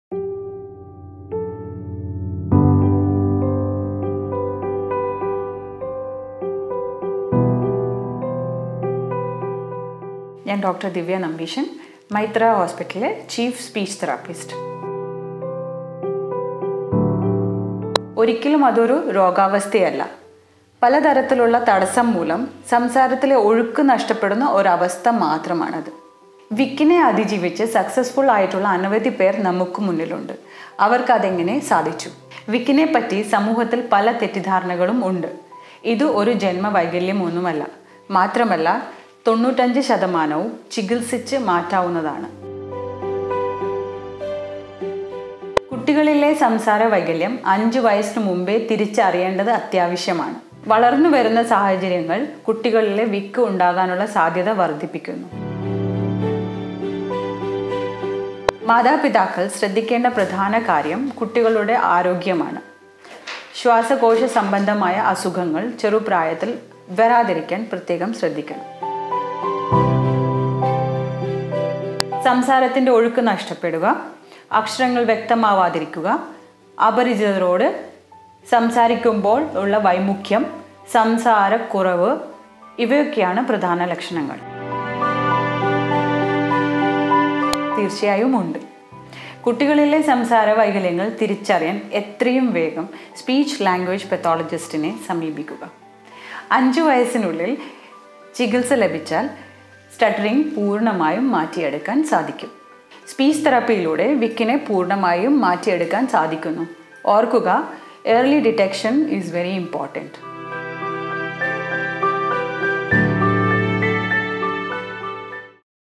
Importance of early detection and management of Stuttering | Dr.Divya | Meitra Hospital
22nd October - International Stuttering Awareness Day

Dr.Divya Nambisan, Chief Audiologist is explaining the importance of early detection and timely management of stuttering.